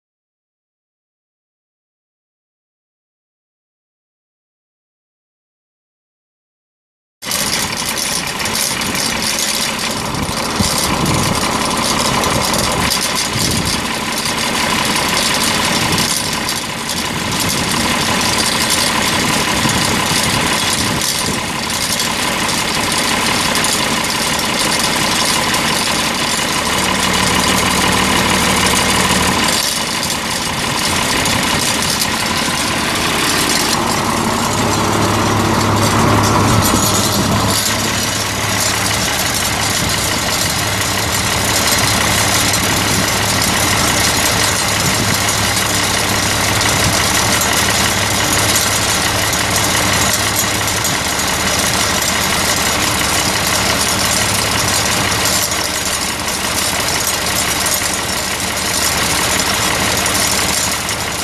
Ag Gator Injactor 2004 self-propelled applicator for sale | no-reserve auction March 23, 2016
View a detailed listing of this item and place bids

Ag Gator Injactor 2004 self-propelled applicator
5,382 miles on meter
1,349 hours on meter
Cummins 6BT 5.9L six cylinder turbo diesel engine
192 HP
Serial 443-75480
Eaton Fuller Road Ranger 10 speed manual transmission
ROPS
Serial 200G290
AC and heat
Non-operational
Suspension seat
Block heater
9'W injector
3,000 gallon injector tank
32" to 34" row spacing
(4) rajectors
6" side outlet
6" rear outlet
Rear hitch for injector
64x34.00-25 NHS flotation tires
No batteries
Serial 2006-540

Location
City of Hugo
Hugo, OK 74743

Item location address: This item is located near Hugo, OK

It's located in Hugo, OK, and we're selling it to the highest bidder regardless of price.